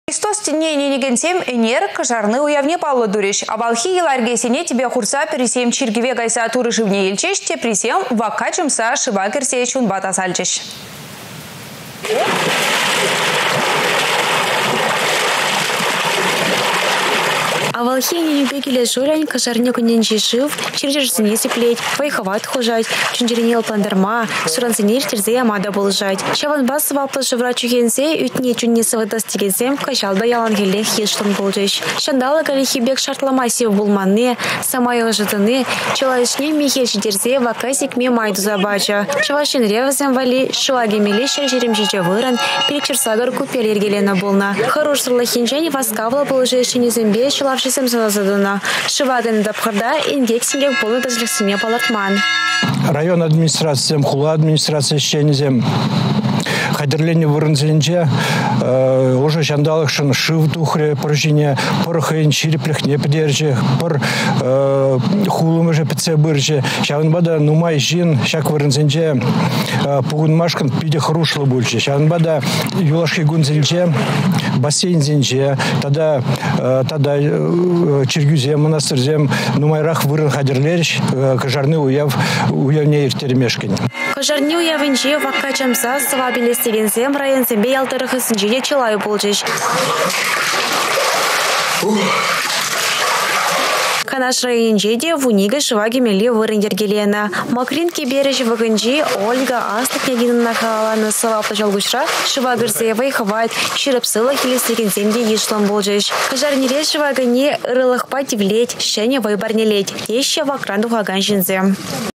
Кăшарни
Христос тĕнне ĕненекенсем  ĕнер « Кăшарни»  уявне паллă турĕç. Авалхи йала-йĕркесене тĕпе хурса пĕрисем чиркĕве кайса Турă шывне илчĕç, теприсем вакка чăмса  шыва кĕрсе чунпа тасалчĕç.